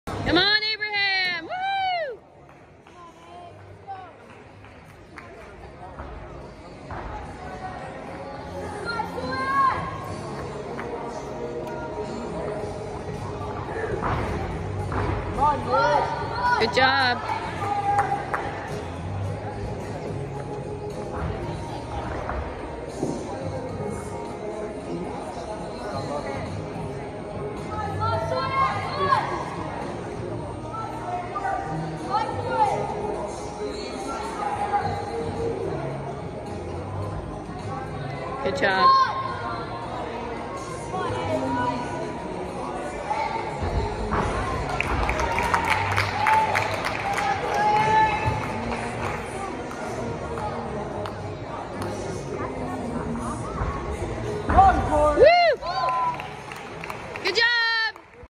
Floor 9 AZ State Gymnastics 2020 ATJ
Abraham performs floor at Arizona State gymnastics tournament level nine. Floor 12.6, T2